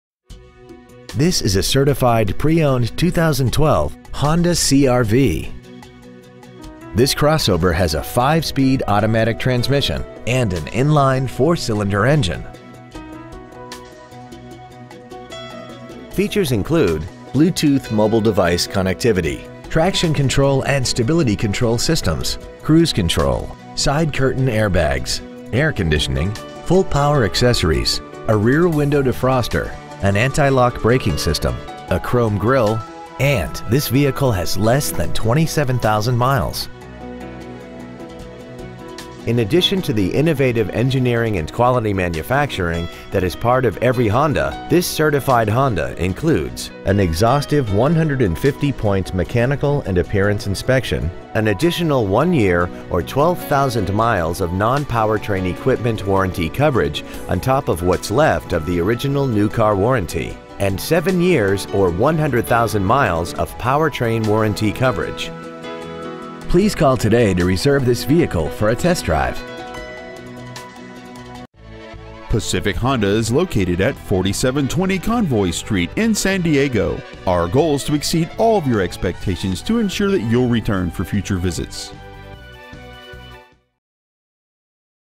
Preowned 2012 Honda CR-V San Diego CA
We have been honored to serve the San Diego CA area , we promise that your experience at our dealership will exceed your expectations ! Year : 2012 Make : Honda Model : CR-V Engine : 2.4L 4 cyls Trans . : Automatic 5-Speed Exterior : Opal Sage Metallic Miles : 26,159 Stock : 143168X Pacific Honda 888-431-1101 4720 Convoy Street San Diego , CA 92111